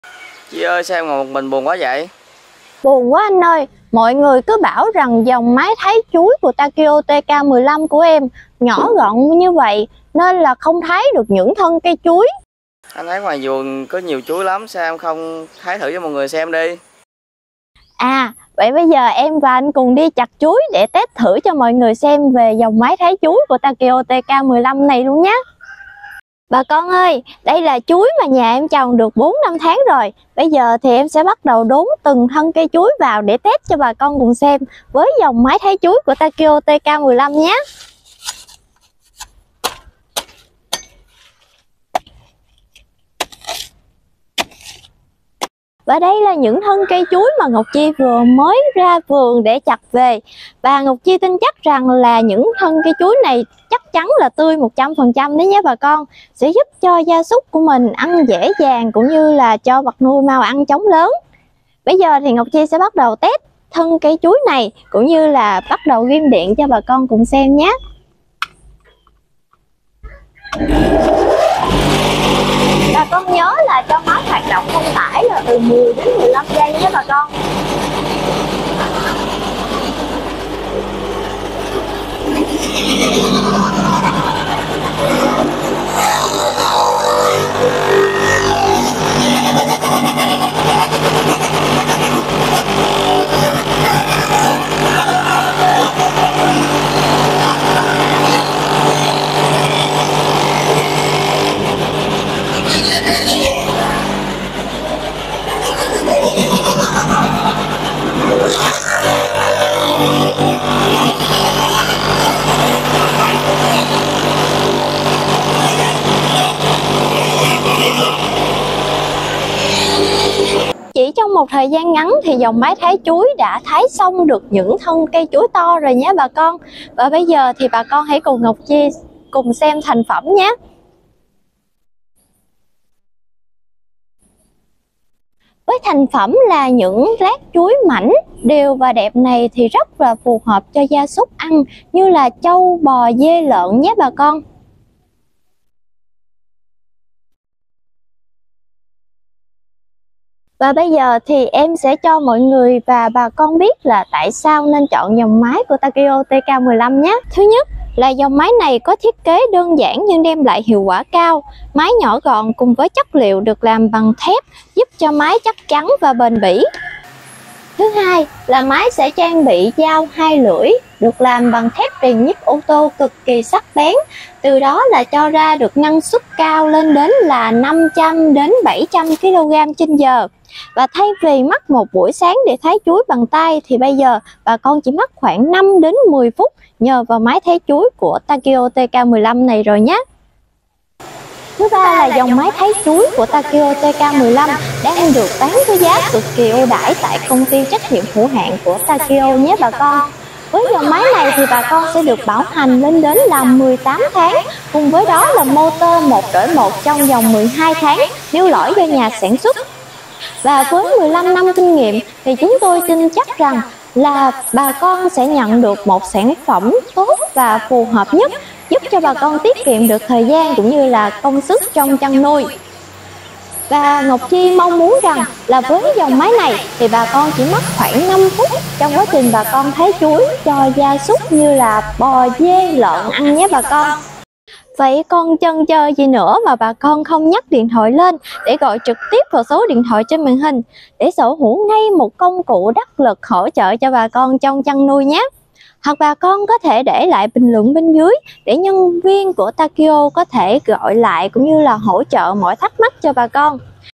Máy thái chuối băm cỏ cho bò dê giá rẻ bảo hành 18 tháng takyo tk 15 dùng điện 1 pha 220v
✅ MÁY BĂM CỎ ĐA NĂNG TAKYO TK 15 GIÁ: 2,300,000 VNĐ
✔️ BĂM cỏ✔️Băm cỏ voi✔️Băm lục bình✔️Băm rau✔️Băm thân cây bắp

 / thegioimay
✅ ƯU ĐIỂM CỦA MÁY THÁI CHUỐI HỖN HỢP TAKYO TK 1.5:
✔️Hệ thống băm được thiết kế phần nạp nguyên liệu rộng hình phễu rất tiện cho các loại rau cỏ cồng kềnh, đặc biệt là cây chuối
✔️Máy sử dụng động cơ 1,5 kw điện 1 pha, thích hợp với các mô hình trang trại có quy mô rộng, vật nuôi đa dạng.
✅LƯU Ý KHI SỬ DỤNG ĐIỂM CỦA MÁY THÁI CHUỐI HỖN HỢP TAKYO TK 1.5:
 ✔️Lắp đặt máy ở nơi bằng phẳng
✔️Khởi động máy sau đó đưa nguyên liệu cần băm vào cửa nạp nguyên liệu, hệ thống dao sẽ làm việc và đưa ra cửa xả nguyên liệu.
✔️Vận hành xong chờ máy gạt hết nguyên liệu ra khỏi máy rồi tắt máy.
✔️Vệ sinh sạch máy sau khi sử dụng xong giúp máy có tuổi thọ cao hơn.
✅THÔNG SỐ KỸ THUẬT CỦA ĐIỂM CỦA MÁY THÁI CHUỐI HỖN HỢP TAKYO TK 1.5:
✔️Công suất động cơ   1.5( kw)
✔️Hai lưỡi giao thái thép rèn công nghiệp